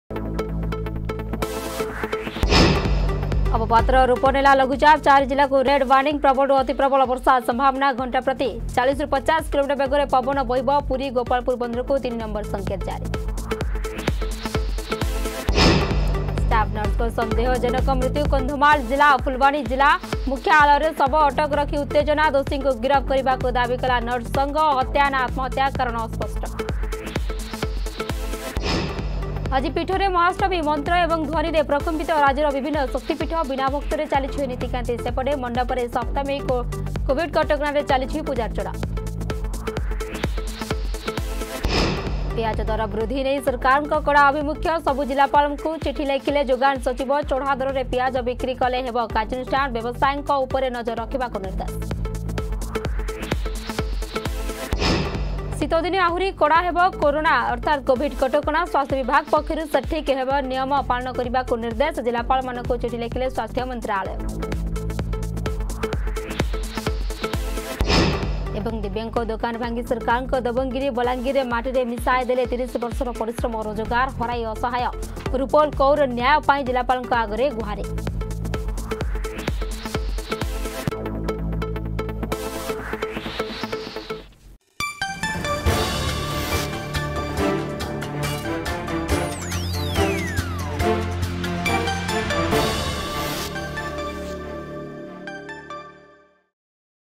Headlines 8Am
ଷ୍ଟାଫ ନର୍ସଙ୍କ ସନ୍ଦେହ ଜନକ ମୃତ୍ୟୁ... ଦିବ୍ୟାଙ୍ଗଙ୍କ ଦୋକାନ ଭାଙ୍ଗି ସରକାରଙ୍କ ଦବଙ୍ଗଗିରି... ଅବପାତର ରୂପ ନେଲା ଲଘୁଚାପ... ଶୀତ ଦିନେ ଆହୁରି କଡା ହେବ କୋଭିଡ କଟକଣା...